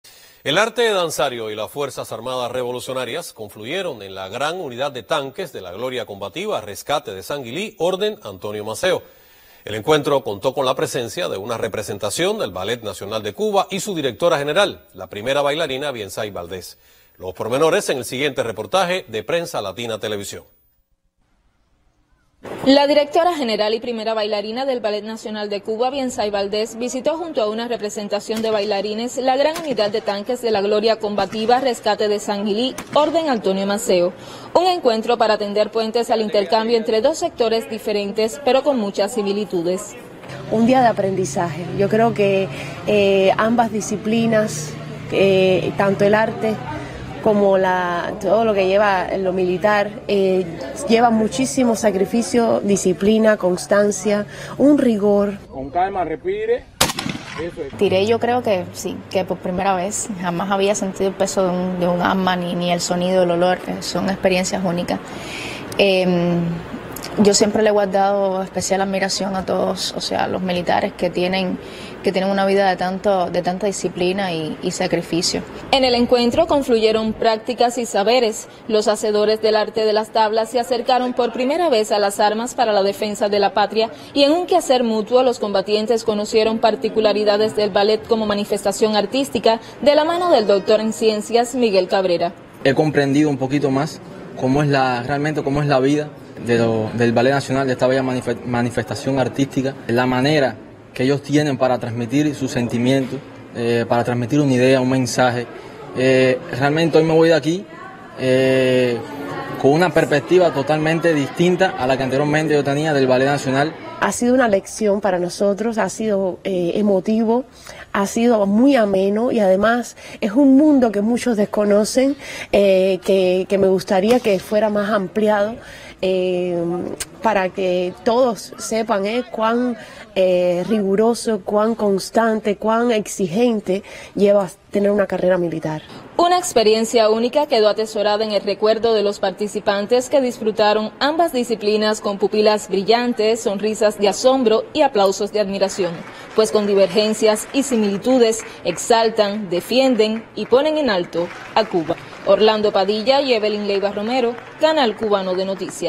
Visitan artistas del Ballet Nacional de Cuba Gran Unidad de Tanques de las FAR
El arte danzario y las Fuerzas Armadas Revolucionarias confluyeron en la Gran UnidadDeTanques de la gloria combativa Rescate de Sanguily, orden Antonio Maceo. El encuentro contó con la presencia de una representación del BalletNacional de Cuba y su directora general, la primera bailarina Viengsay Valdés. Los pormenores, en el siguiente reportaje de Prensa Latina Televisión.